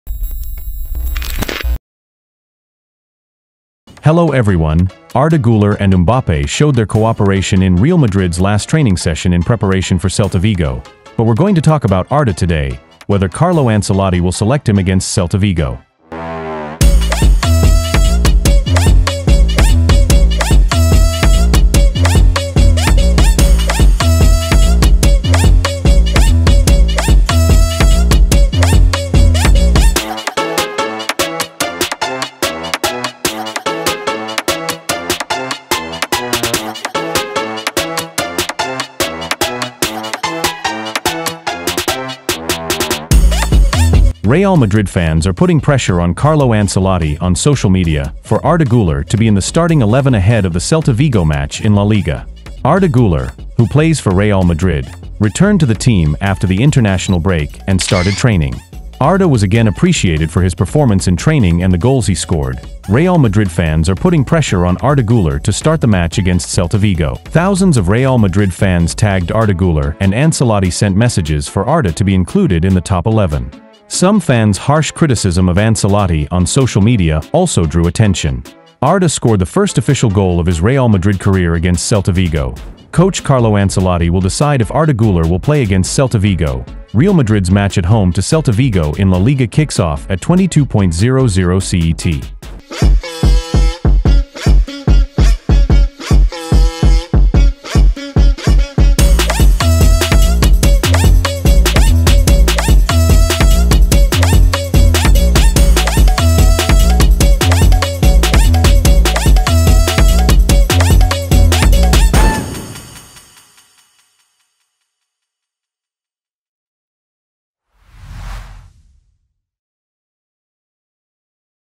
ARDA GULER'S COOL PASS TO MBAPPE BEFORE THE CELTA VIGO MATCH HERE'S CARLO ANCELOTTI'S REACTION!!
🚨NEWS:
Arda Guler and Mbappe showed their cooperation in Real Madrid's last training session in preparation for Celta Vigo, but we're going to talk about Arda today, whether Carlo Ancelotti will select him against Celta Vigo.

Real Madrid fans are putting pressure on Carlo Ancelotti on social media for Arda Güler to be in the starting 11 ahead of the Celta Vigo match in La Liga. Arda Güler, who plays for Real Madrid, returned to the team after the international break and started training. Arda was again appreciated for his performance in training and the goals he scored, Real Madrid fans are putting pressure on Arda Güler to start the match against Celta Vigo.